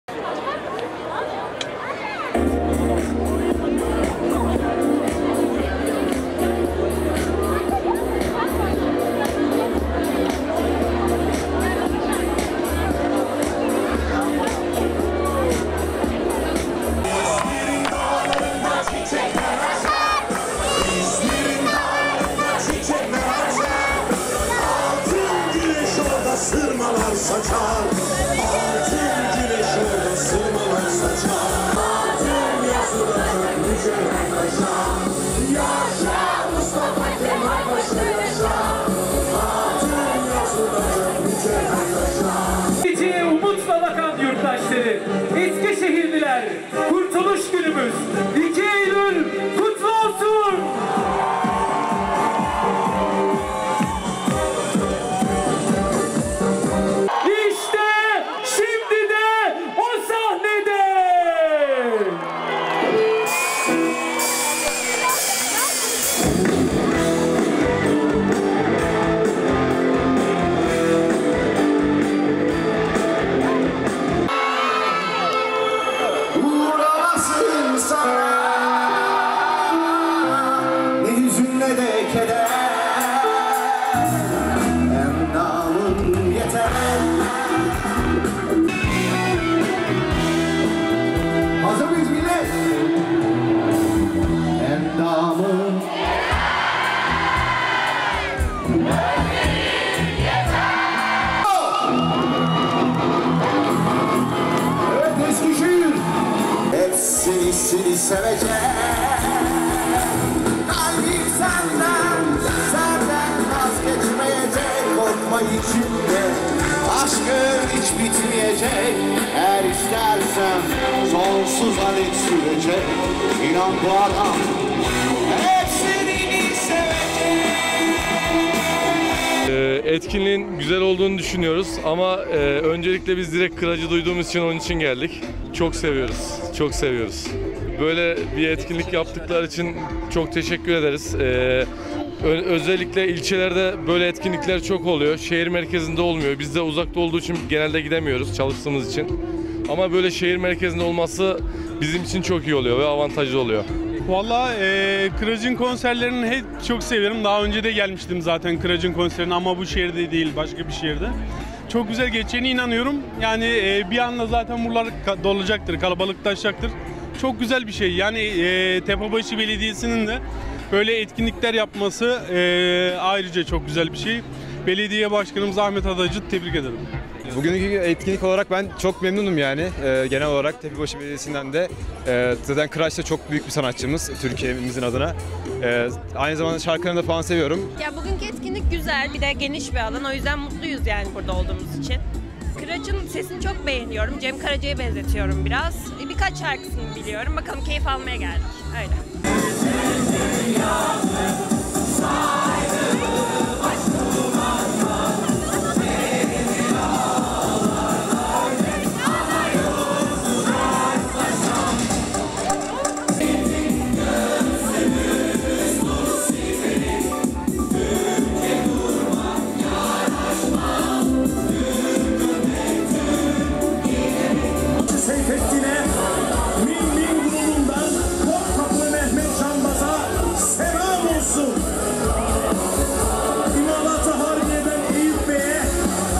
KURTULUŞ COŞKUSU KIRAÇ İLE YAŞANDI
Tepebaşı Belediyesi, Eskişehir’in düşman işgalinden kurtuluşunun 100’üncü yıl dönümü dolayısı ile sevilen sanatçı Kıraç Eskişehirlilerle buluştu